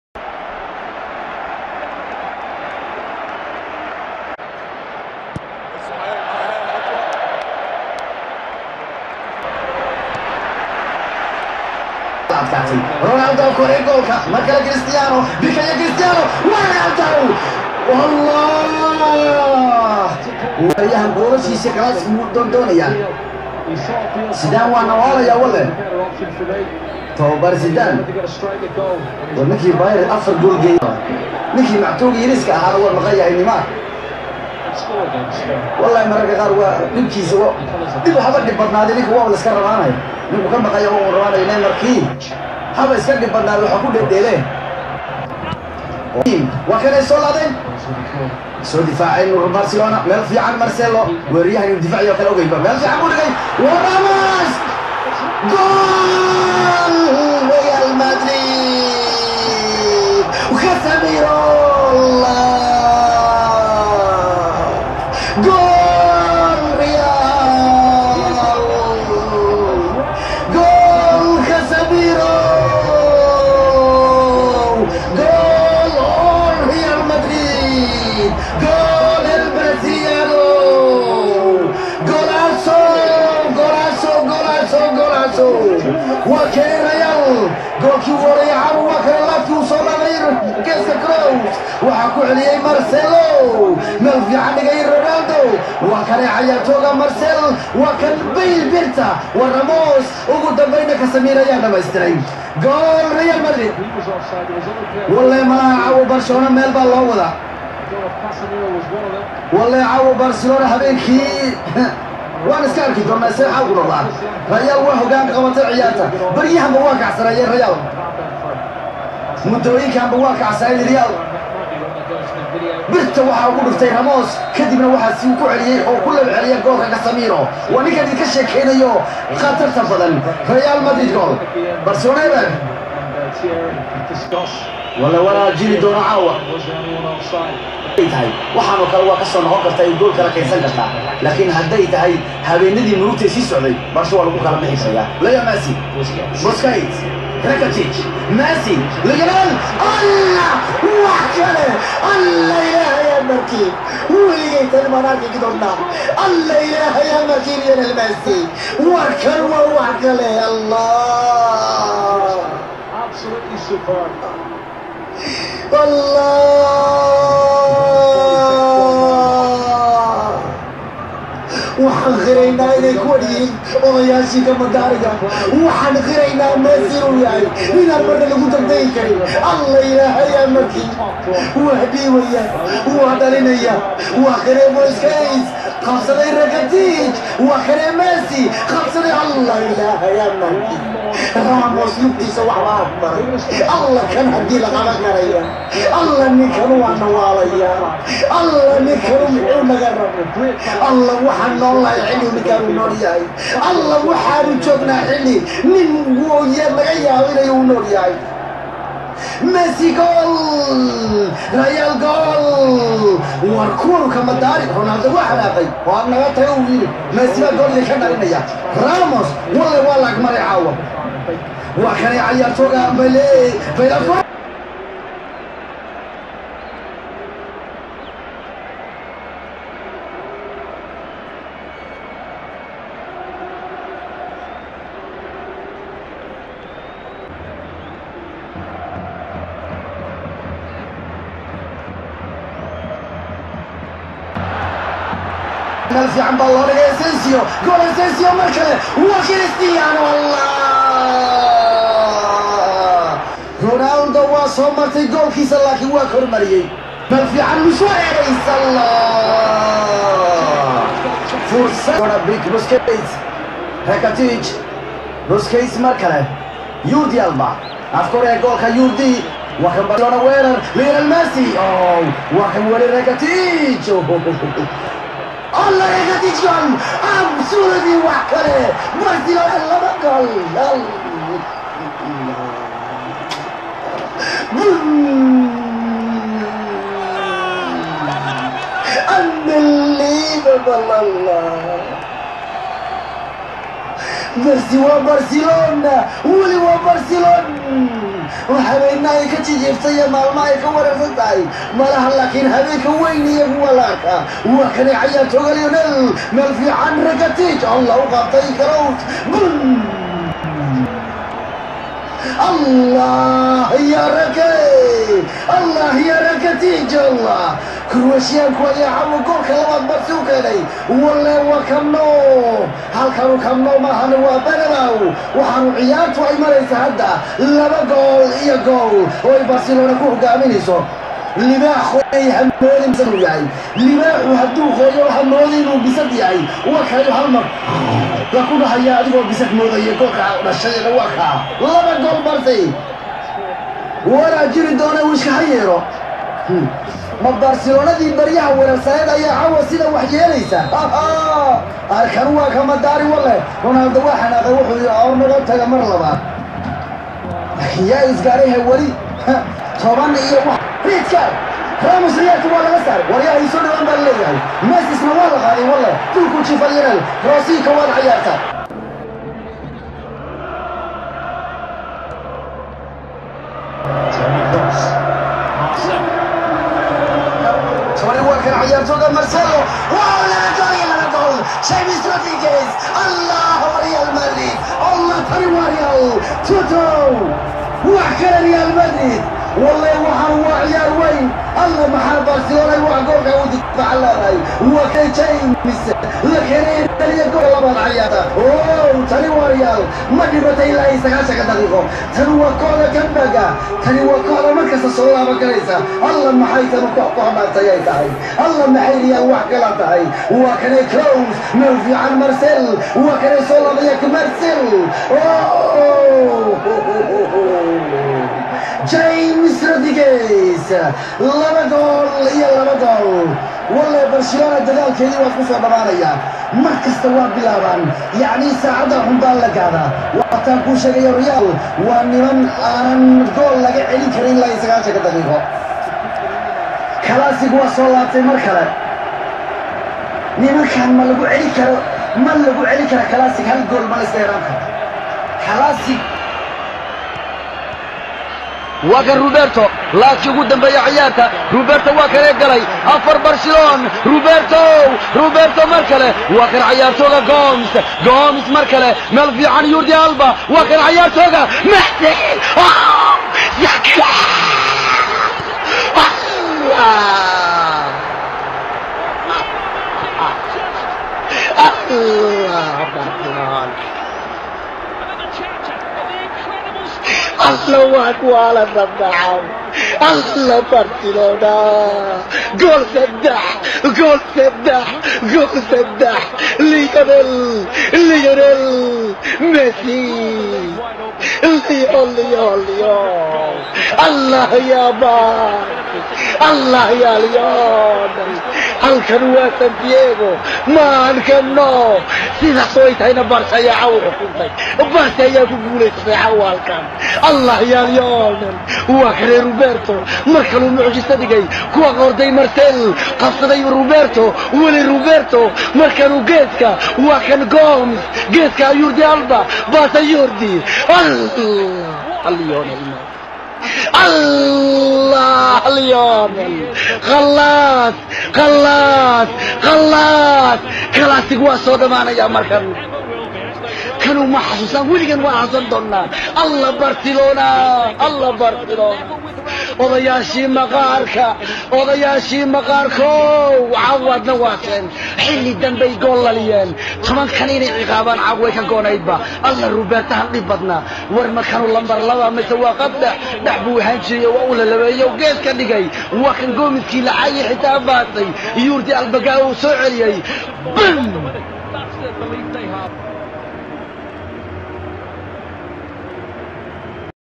Real Madrid 🆚 barcelona 2-3 | Mohamed qadar goos goos
Tabiyaha Ciyaarta wariye Mohamed Qadar

Fadlan SUBSRIBE DHEH SI AAD NOOLA SOCOTO